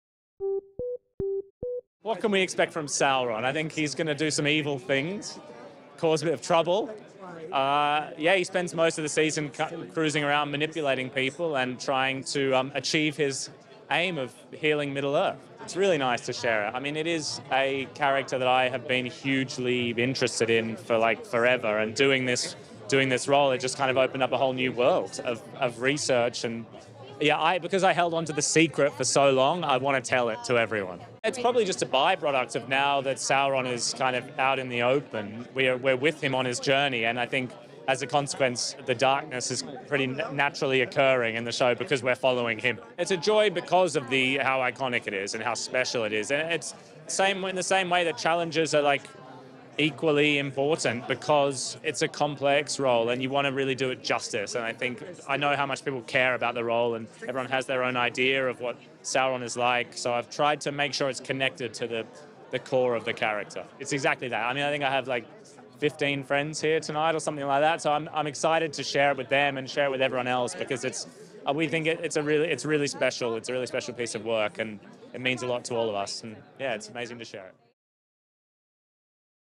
Charlie Vickers interview on The Lord of the Rings: The Rings of Power Season 2
Charlie Vickers presents his insights on "The Lord of the Rings: The Rings of Power Season 2," hinting at his character's darker journey. He discusses the joy and challenges of portraying such an iconic role, emphasizing the importance of staying true to Sauron's essence. Vickers shares his excitement about finally revealing his character's evolution, reflecting on the natural emergence of darkness as the narrative unfolds. He also touches on the personal significance of the project, expressing eagerness to share this special work with friends and audiences worldwide.